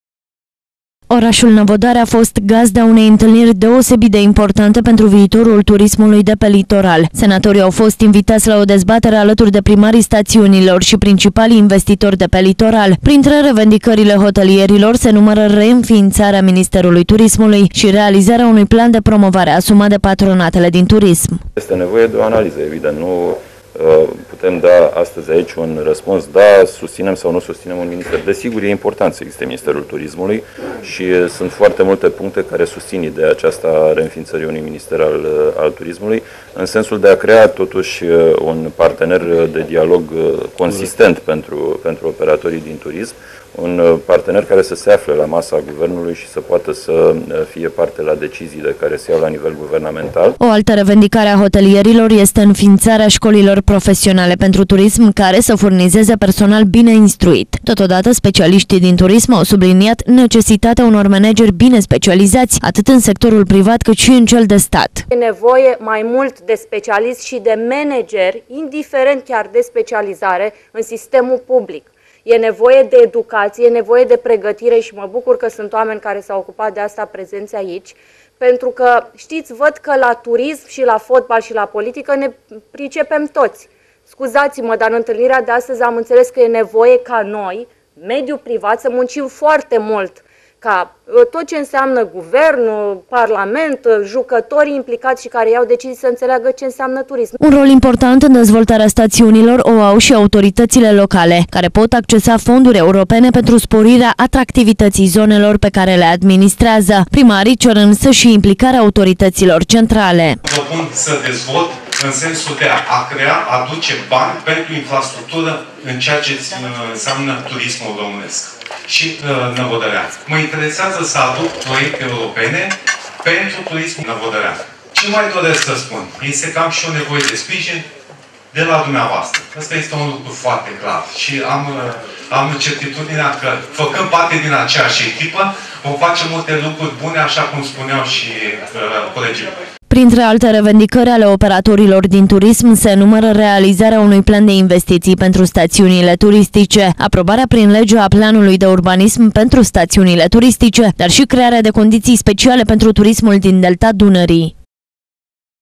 Știre Etv - Întâlnire importantă pentru viitorul turismului | 16.06.2016
Confruntare de gradul zero pe litoral. Hotelierii s-au întâlnit cu senatorii și cu primarii stațiunilor pentru rezolvarea principalelor probleme de care se lovește industria ospitalității. Printre ele se numără înființarea școlilor profesionale pentru turism, dar și administrarea stațiunilor, în parteneriat public-privat.